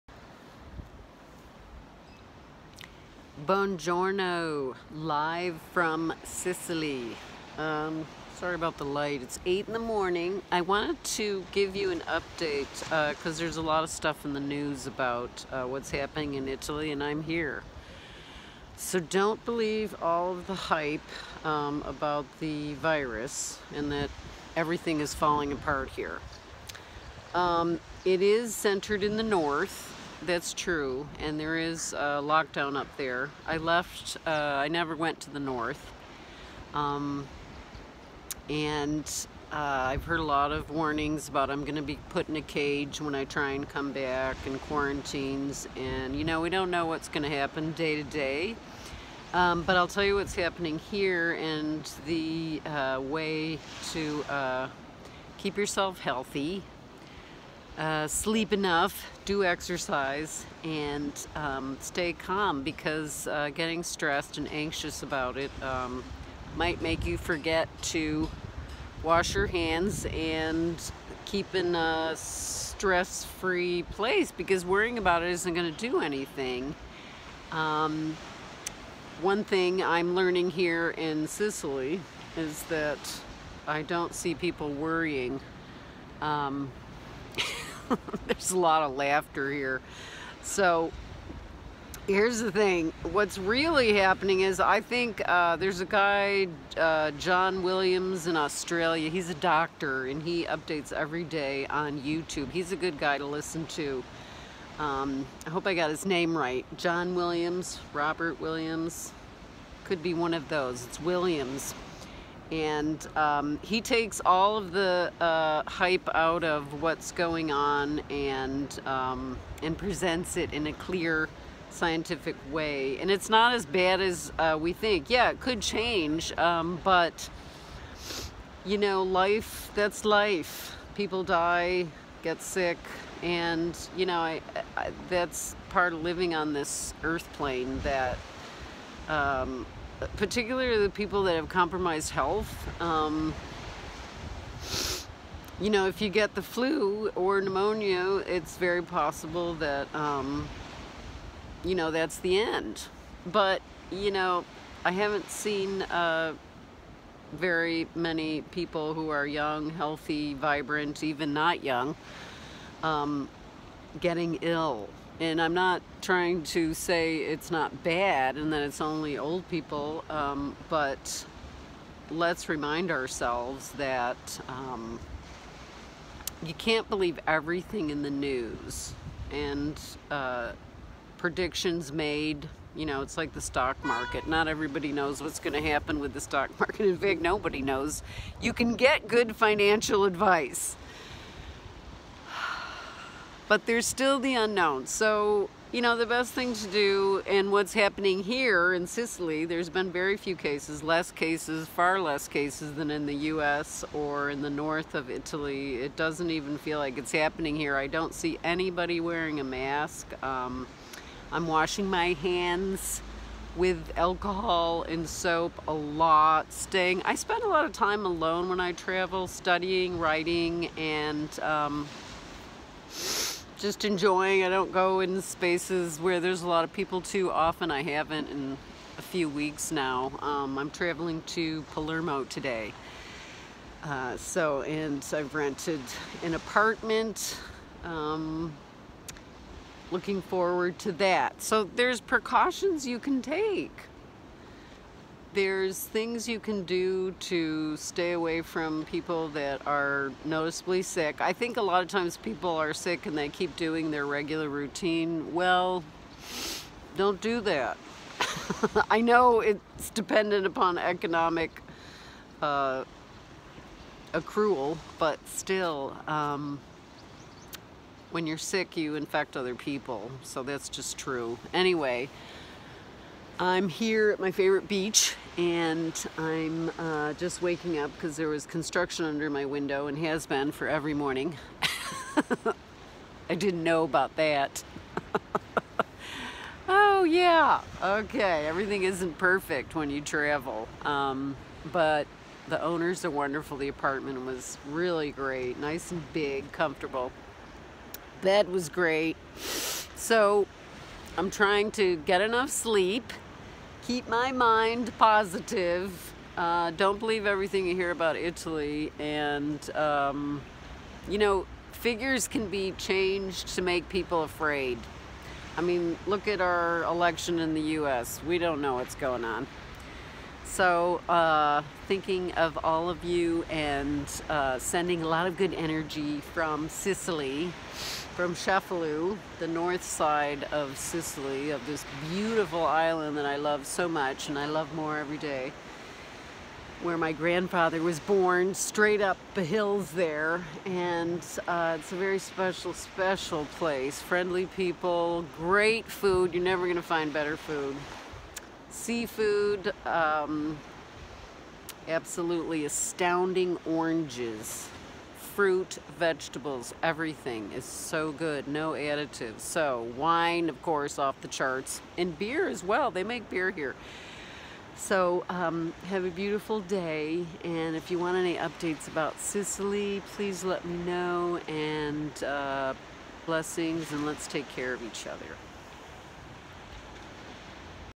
Coronavirus Italy
I'm in Sicily and have been in Italy for 7 weeks, I was not in the North where the most cases are reported but I wanted to give you a realistic picture of life in Italy and a perspective that doesn't rely on media hype.

Author's 5 books on Amazon

What I Learned from 11 Years of Solo Travel